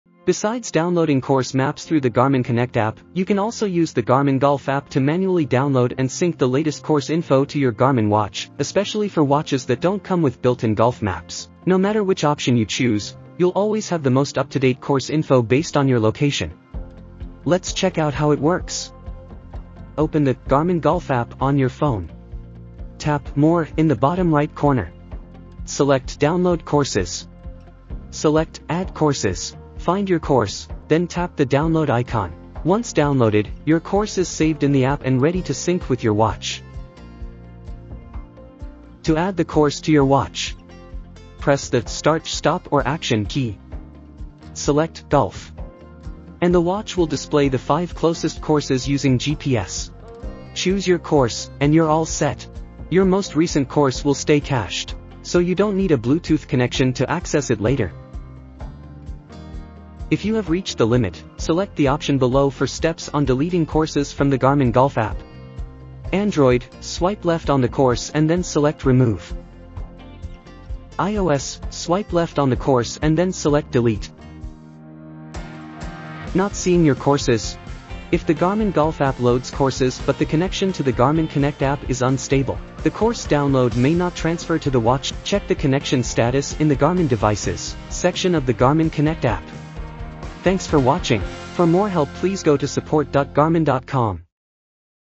Tutorial – Downloading Golf Courses to Your Watch Using the Garmin Golf App
Learn how to manually download and sync the latest golf course maps to your Garmin watch using the Garmin Connect and Garmin Golf apps. Stay updated with the most accurate course info no matter your location, even if your watch doesn’t have built-in golf maps!

▶Timecode:
0:00 – intro
0:14 – HOW TO DOWNLOAD GOLF COURSES
0:38 – ADDING COURSES TO YOUR WATCH
1:02 – DELETING COURSES FROM GARMIN GOLF APP
1:20 – TROUBLESHOOTING TIPS

▶For Additional Assistance:
for more detailed support and FAQs.